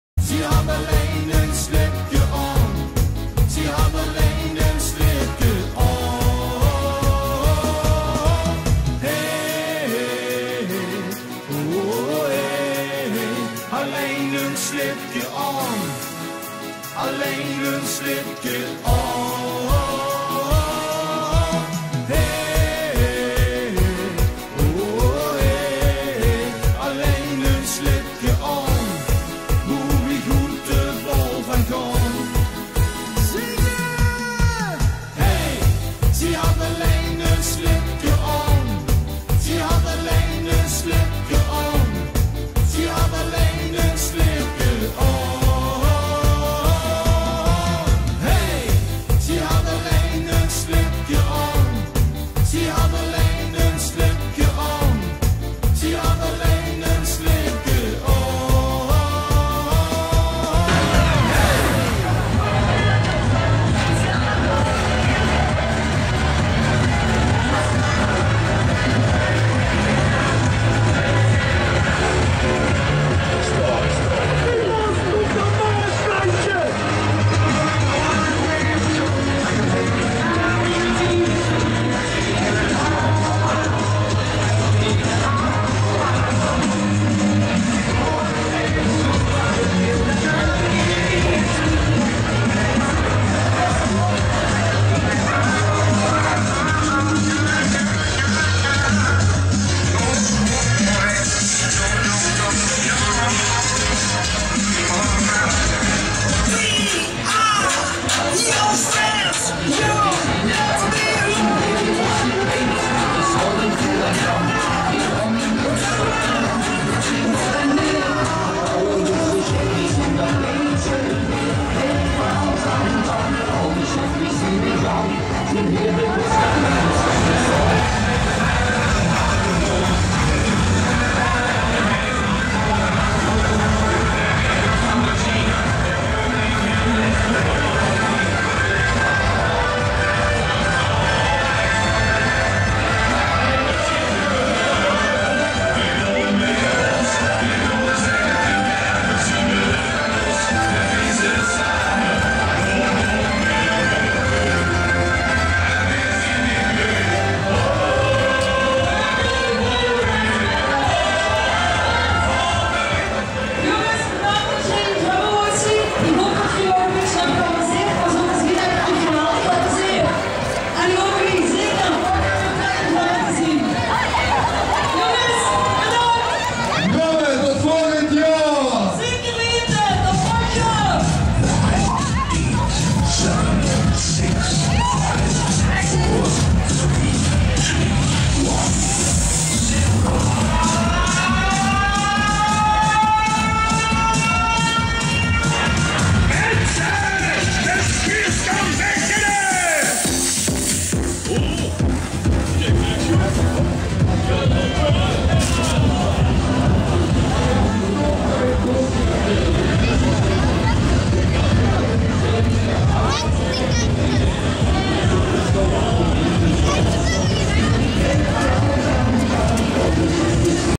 KV Sheif a/d Maas Ukeve & Grümme 2013
Kv Sheif a/d Maas tijdens de stoeten Uikhoven & Opgrimbie 2013, wat foto's en filmpjes